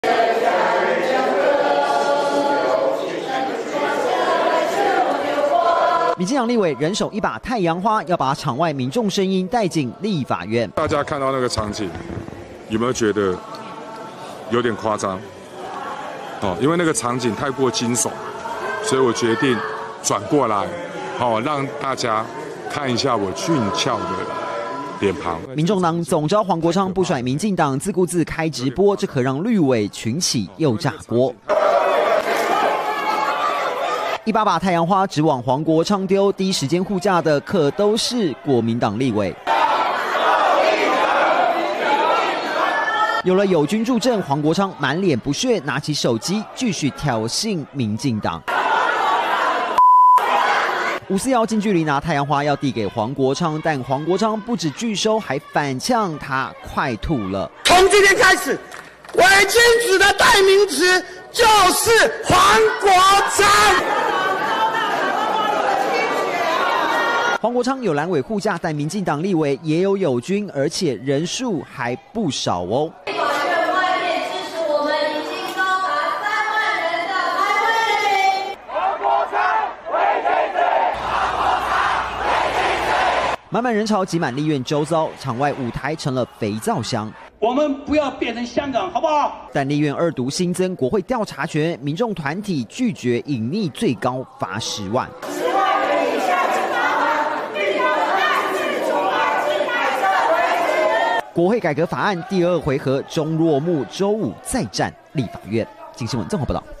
綠委「太陽花」滿天飛！狂嗆黃國昌 藍委護駕｜鏡新聞
立法院昨（5/21）晚續審國會改革方案，場外抗議群眾也越來越多，民進黨表示有三萬人包圍立法院，而場內民進黨立委王世堅，則是狂嗆民眾黨總召黃國昌是「偽君子」，民進黨立委還將太陽花帶進立法院，狂嗆黃國昌。